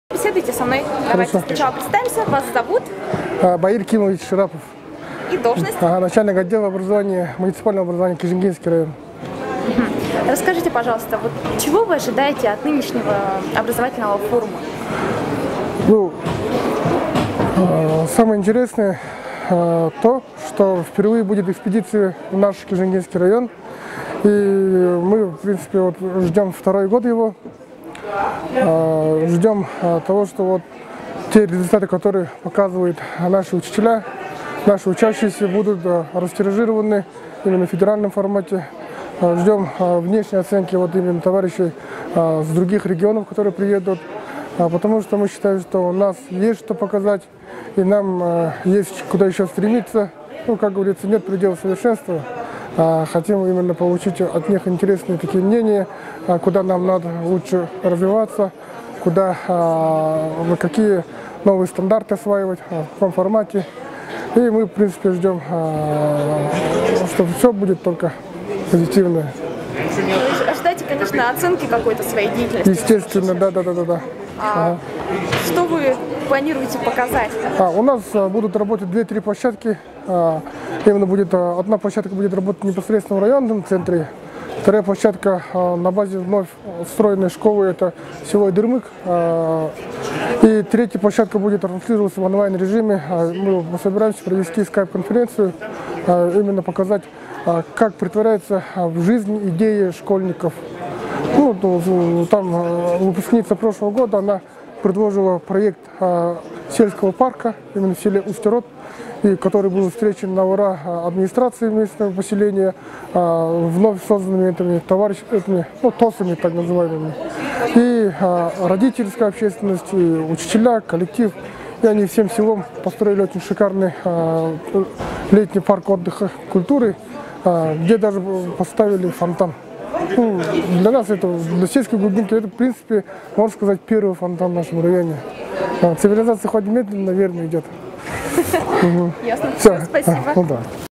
Байкальский образовательный форум 2013
Б.К.Ширапов
Начальник отдела образования МО "Кижингинский район"